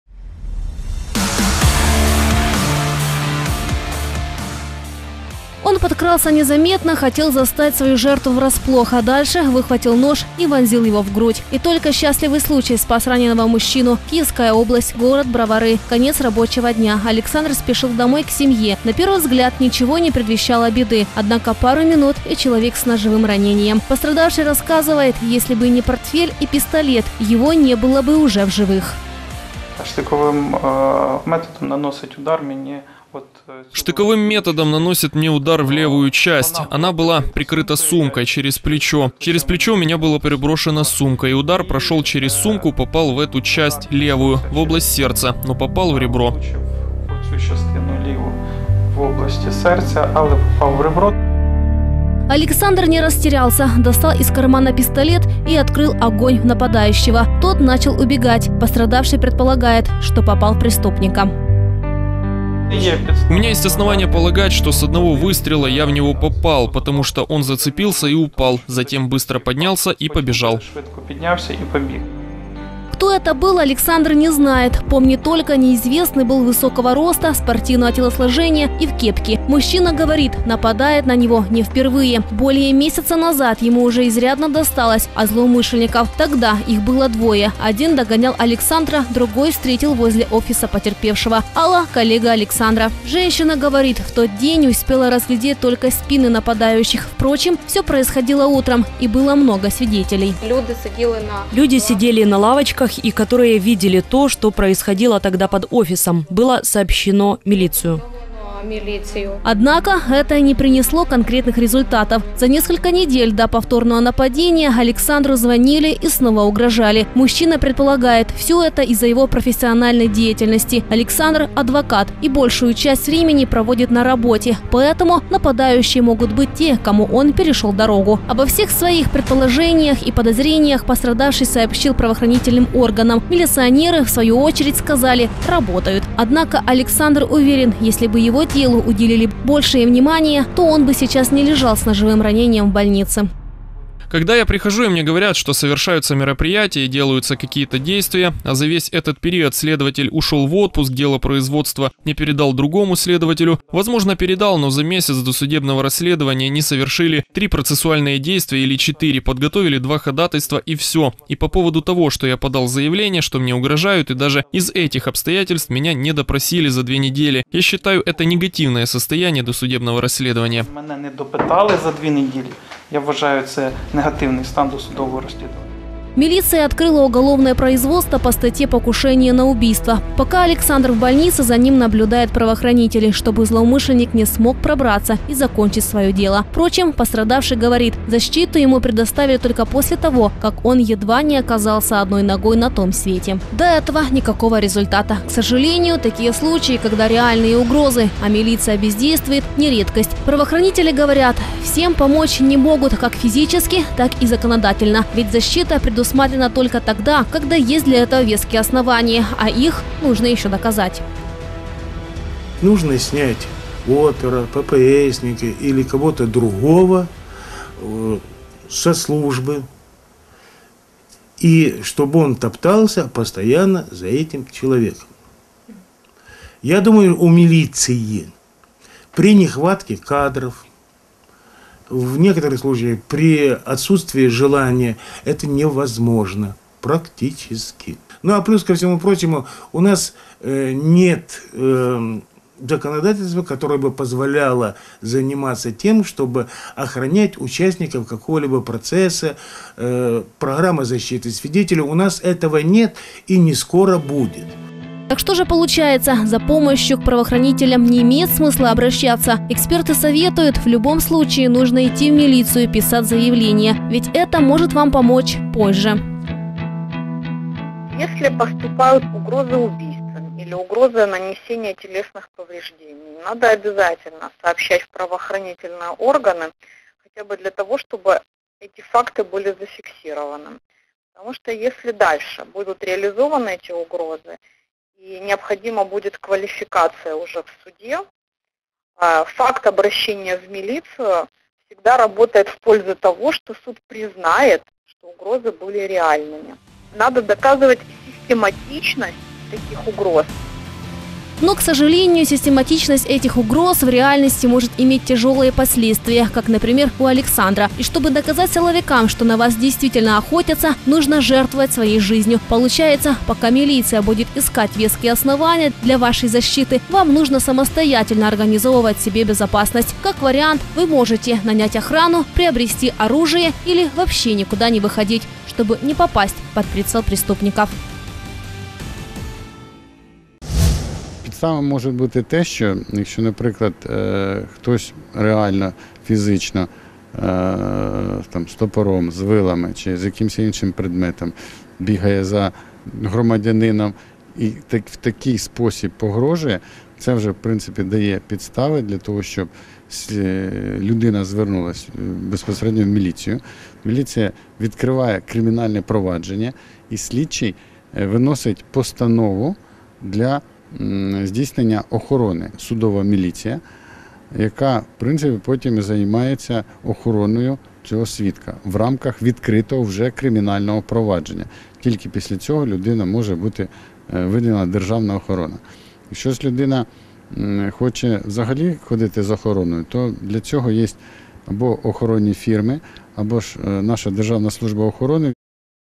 У Київській області намагалися вбити адвоката
У Київській області, в місті Броварах невідомий з ножем  напав на чоловіка і проштрикнув йому груди. Потерпілий дивом врятувався. Дістав пістолет і почав стріляти у нападника. Той накивав  п’ятами. Міліція відкрила кримінальне провадження.